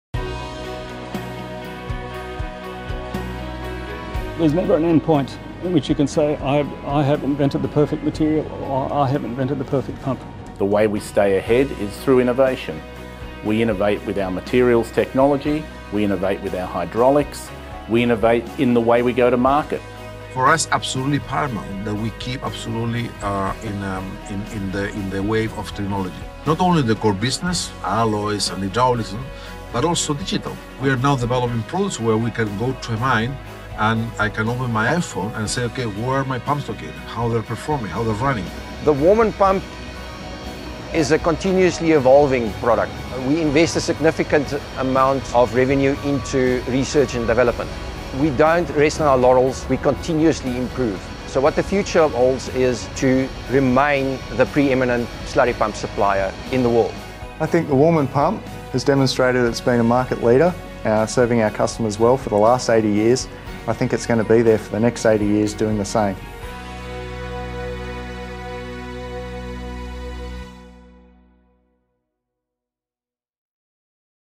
The innovation and technology powering Warman® slurry pumps
Designed for heavy duty, highly abrasive applications, industry leading Warman® centrifugal slurry pumps have been at the forefront of technology for eighty years.

Watch our video to learn how our commitment to innovation and investment into research and development is pushing the boundaries and paving the way for the future. Discover more about Warman® centrifugal slurry pumps